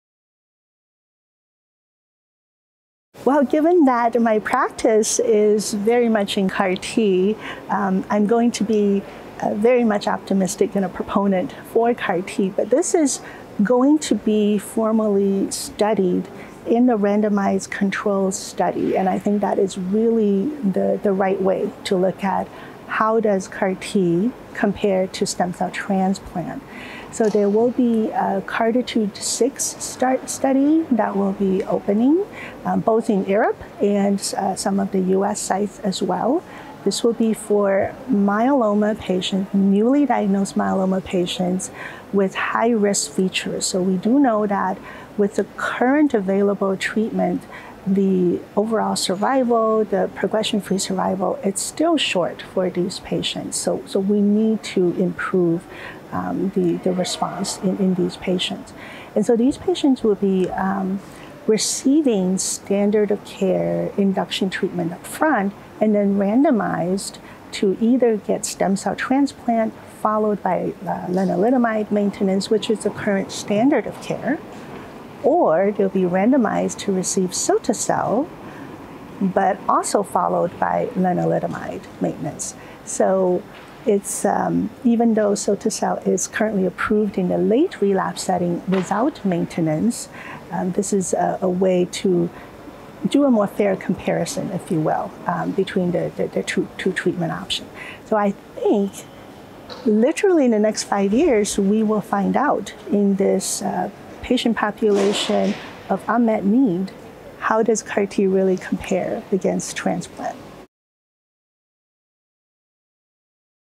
How does CAR-T therapy compare to stem cell transplantation?
Yi Lin, MD, PhD, Mayo Clinic, Rochester, MN, shares some insights into the role of CAR-T therapy versus stem cell transplantation (SCT) in multiple myeloma and whether transplant will be replaced in the next five years. Dr Lin emphasizes the importance of the upcoming CARTITUDE-6 trial (NCT05257083), which will be comparing induction therapy followed by SCT and lenalidomide maintenance versus induction therapy followed by ciltacabtagene autoleucel (cilta-cel) and lenalidomide maintenance in patients with high-risk multiple myeloma, and will answer some very important questions in the field. This interview took place at the European Hematology Association (EHA) Congress 2022 held in Vienna, Austria.